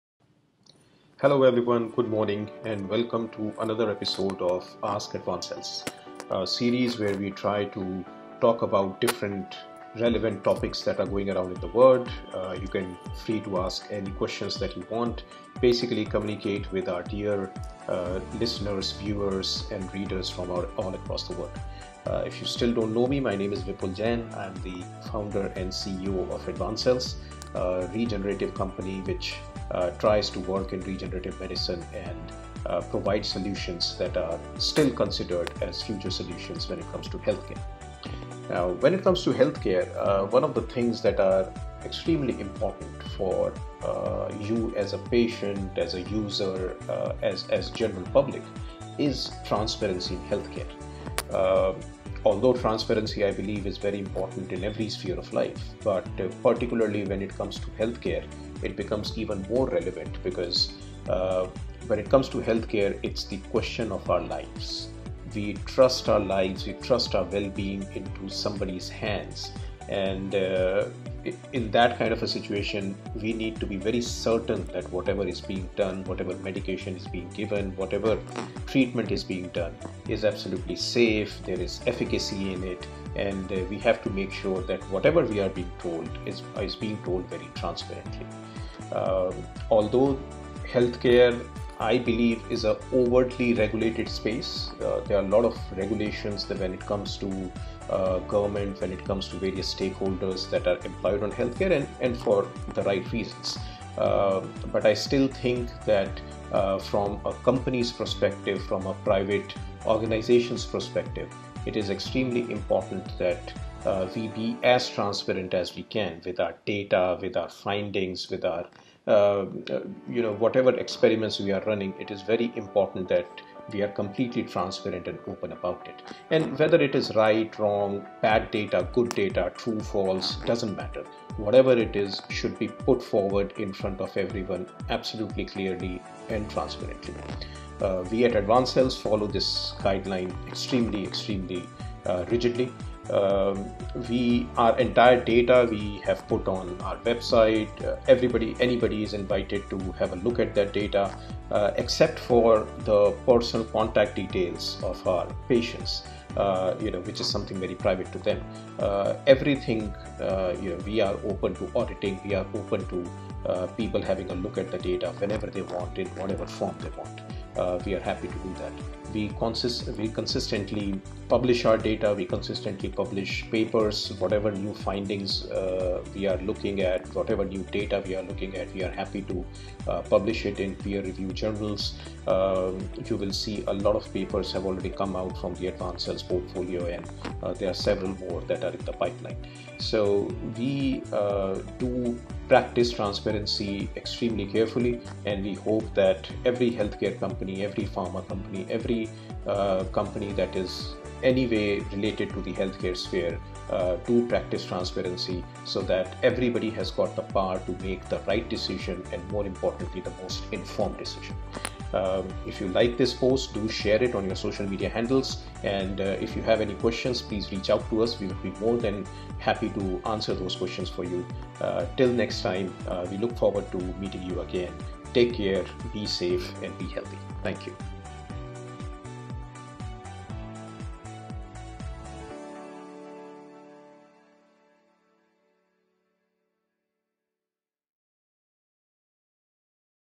How to Keep Transparency in HealthCare | AskAdvancells​ EP14
In health care today, the conversation around transparency centers on the patients. The patient is empowered to ask for treatment options and costs, potential treatment risks, realistic outcomes, and much more. Health care providers must respond with as much information as possible to ensure appropriate care is delivered, quality and safety are top of mind, and patients and their care team can make thoughtful care decisions. When team members are open and honest with each other, without fear, it leads to mutual trust, collaboration, and sharing of best practices across disciplines. Always remember-Patients are the ultimate beneficiaries.

Join Mr. Vipul Jain (CEO, Advancells) on Friday, 19th February at 3:00 PM IST on Facebook Live Broadcast where he will be talking about the importance of transparency in Healthcare and how to keep it for the benefit of patients.

Join AskAdvancells Facebook Live Broadcast and ask your question live on Facebook and we will try to answer every question.